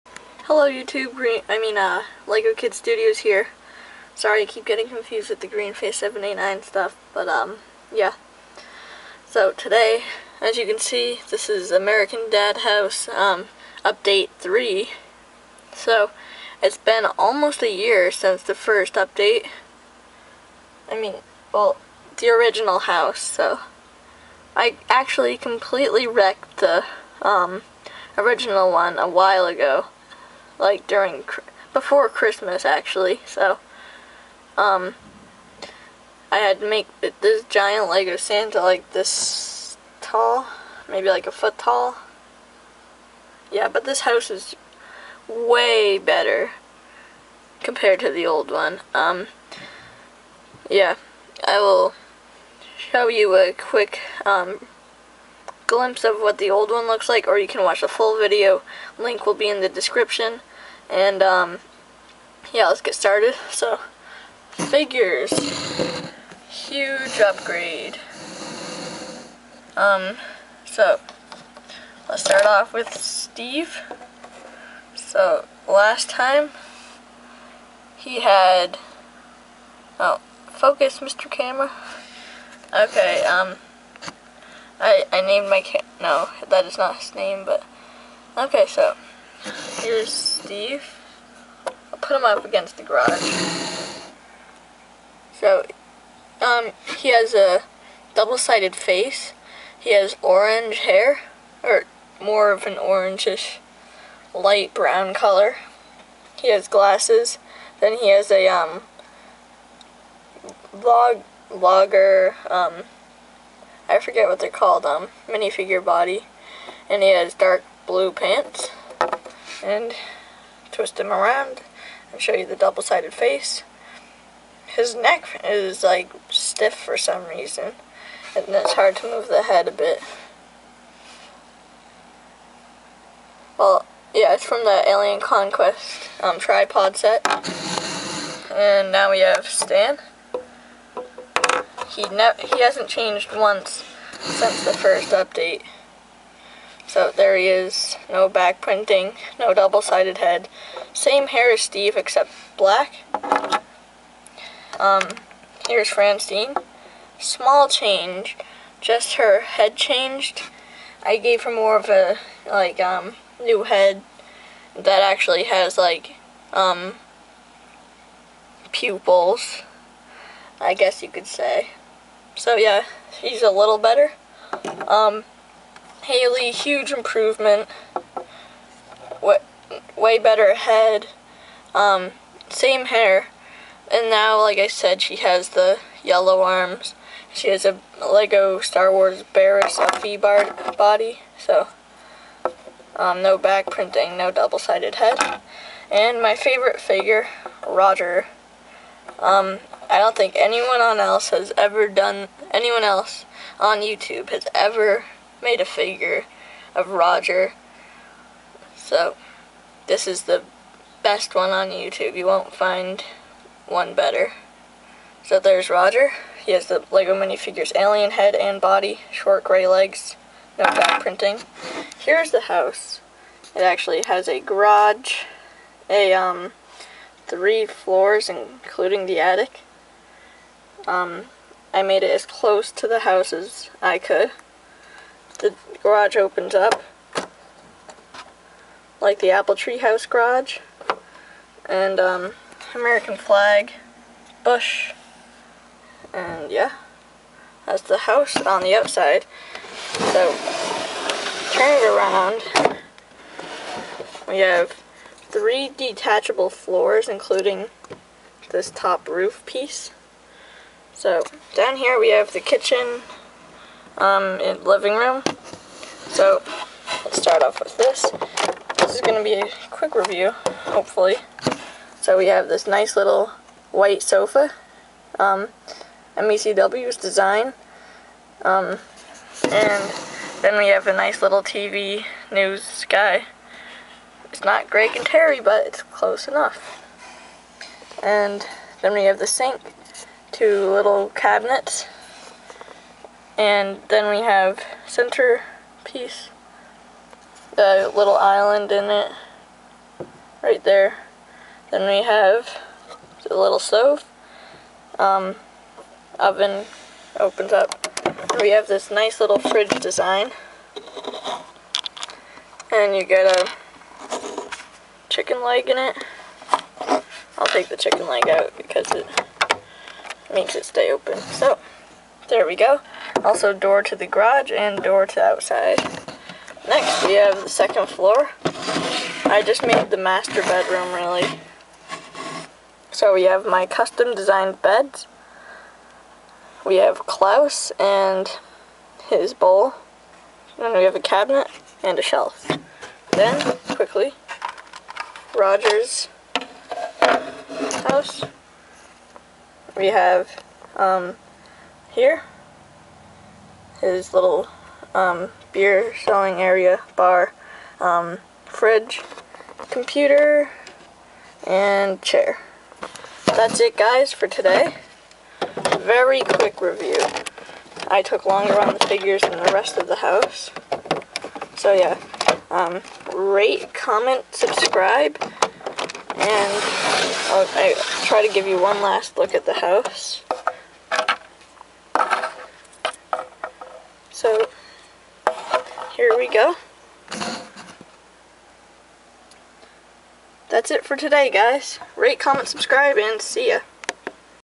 LEGO American-Dad House Review version 1.2
Version 1.2 of the Smith house. Now with updated Steve, Haley, Francine, and NEW Roger! Much more accurate according to the show. link to the old ones here- Update 1